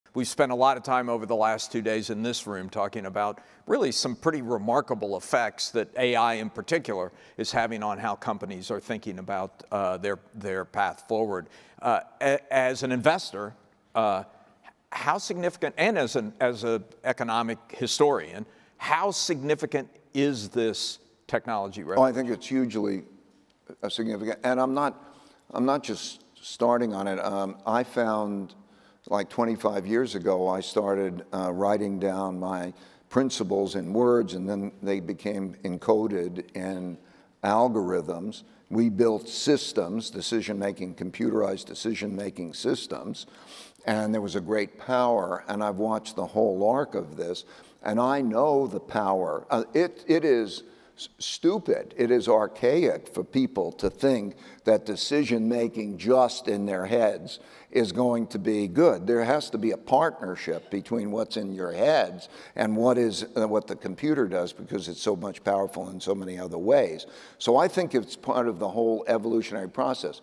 My Thoughts on the Significance of AI
My thoughts on the significance of AI. From my conversation at the @fortune Global Forum.